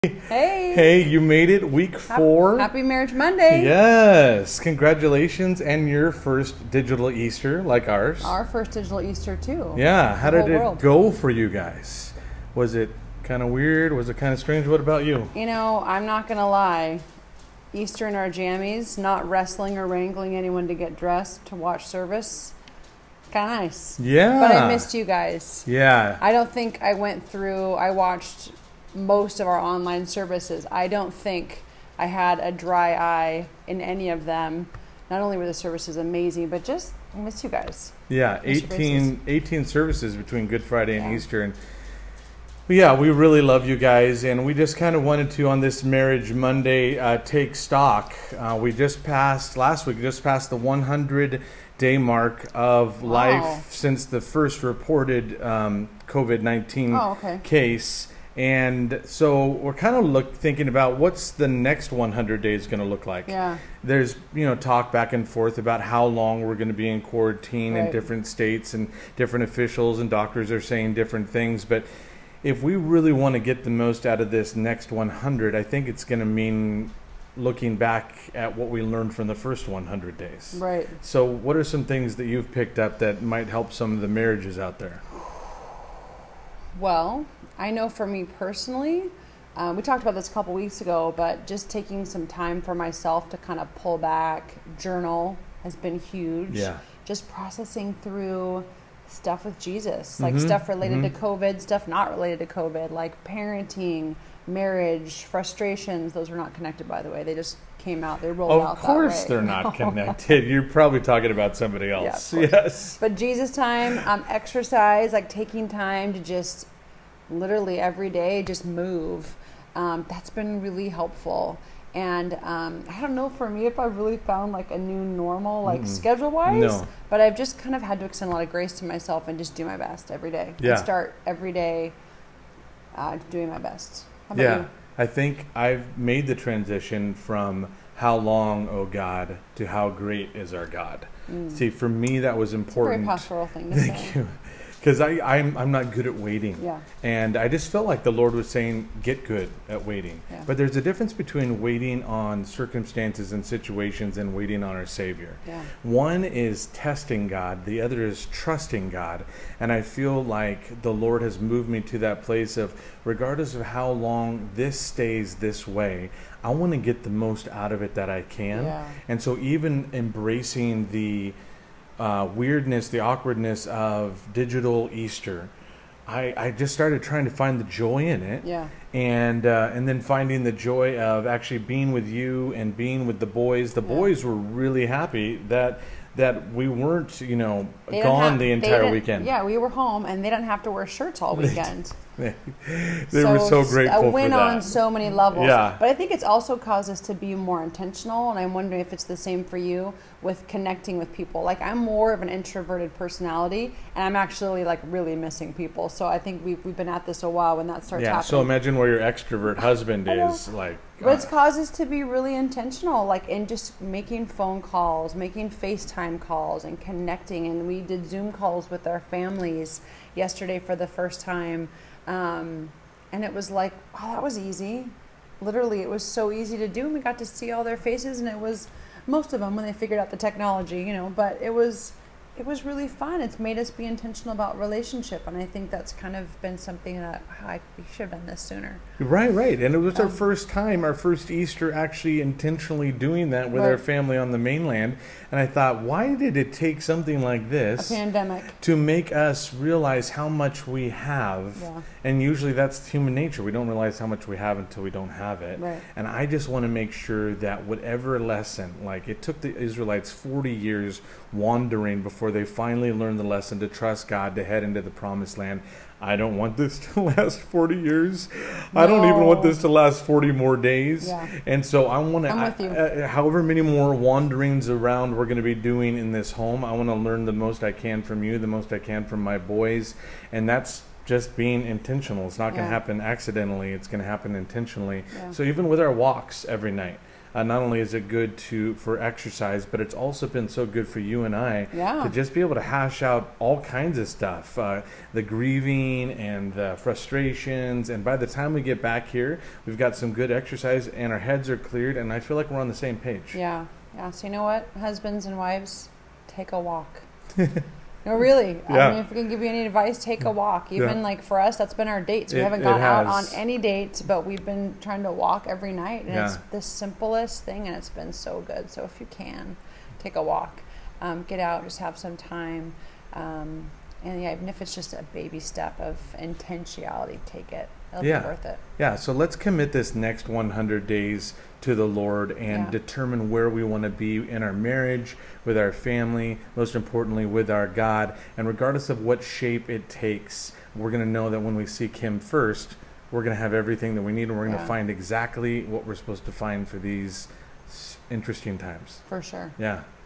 Marriage Mondays: How Long Is This Going To Take?
Week 4 of the Covid-19 Quarantine.  We talk through how to keep our marriage strong in the Lord during these crazy times.